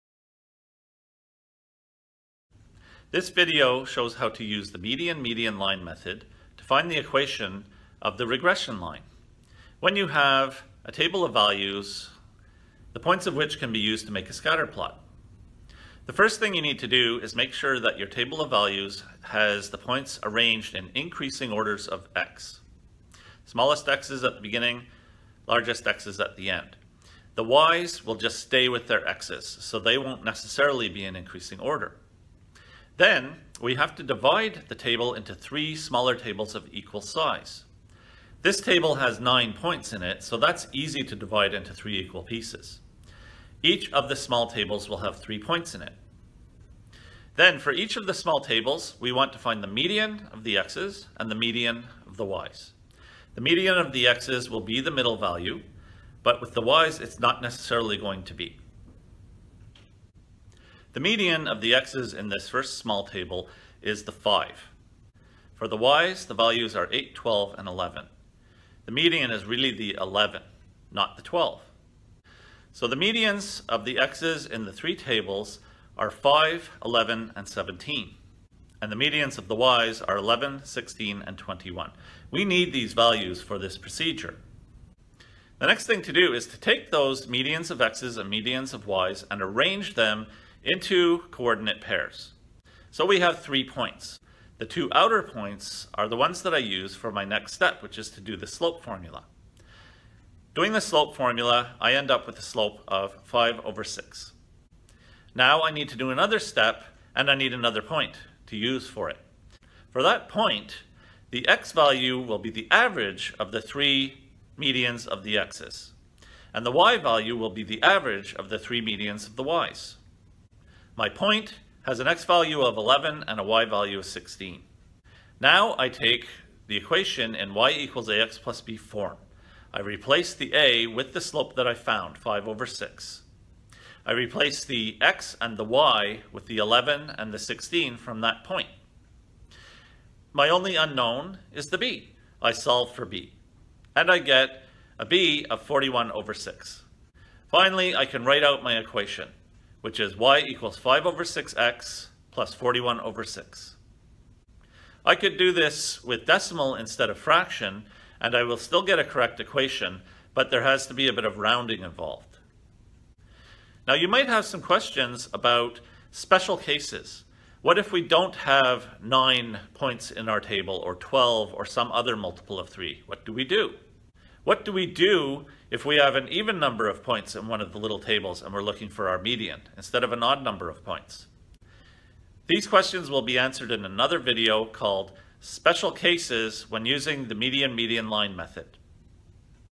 The Median Median Line Method
An intro to the Median Median Line method for finding the equation of a regression line.